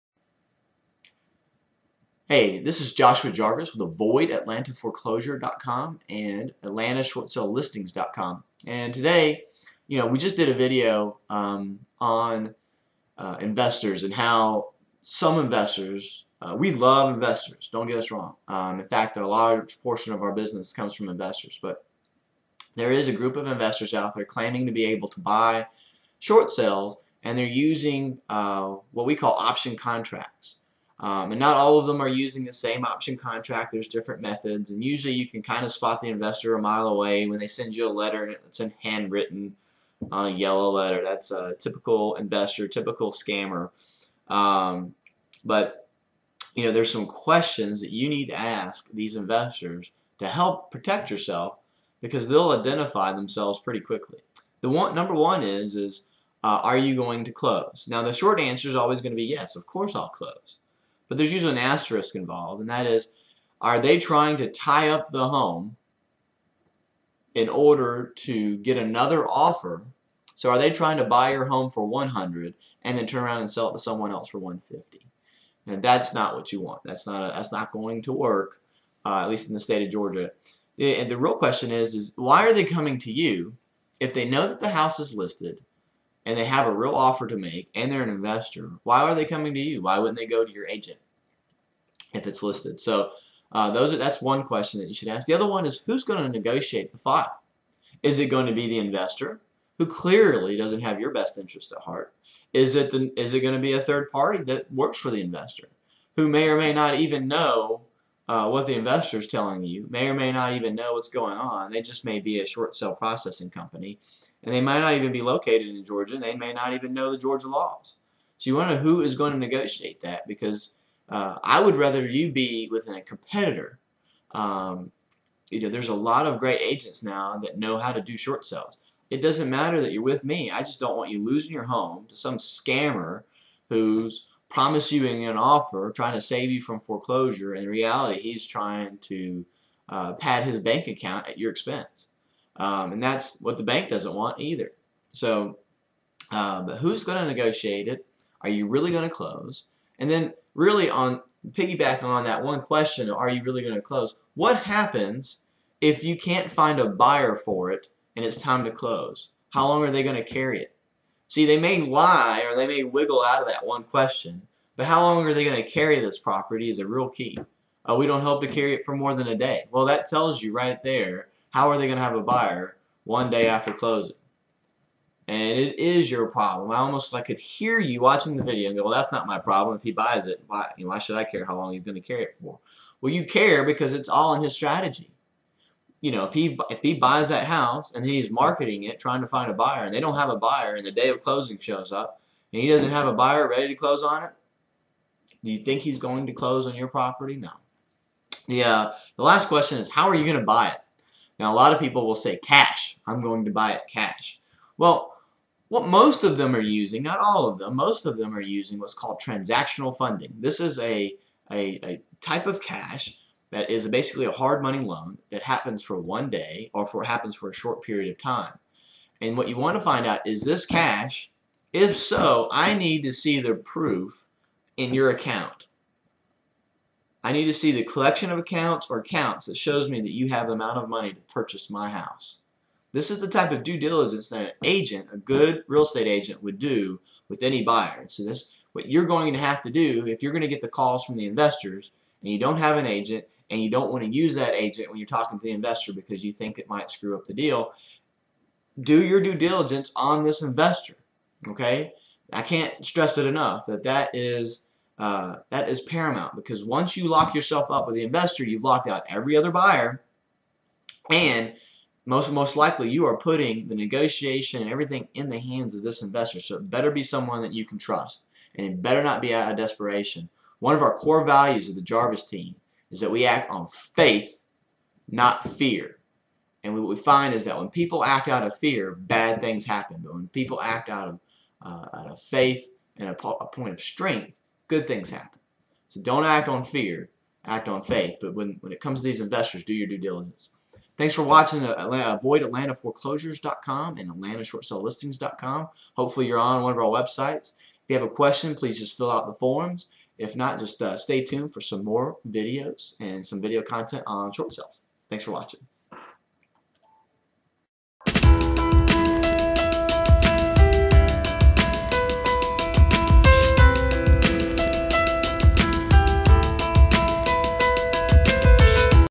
Short Sale Investor Questions
- After mentioning the investors I wanted you to know how to help "qualify" the good ones from the pretenders.